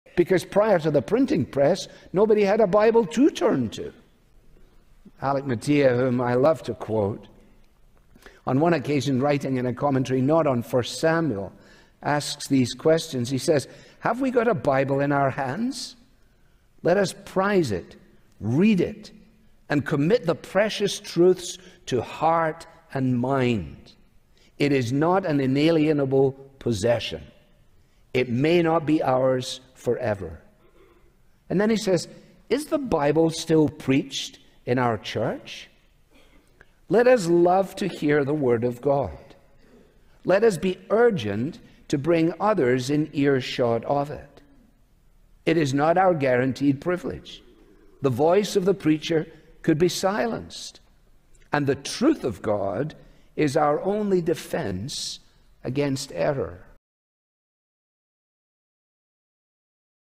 Alistair Begg-The World Needs The Word! Must Watch!
Alistair Begg has a quick word for us to realize. What a blessing to own and store the Word of God in our hearts and homes.
PLEASE SHARE TO OTHER MEDIA OUTLETS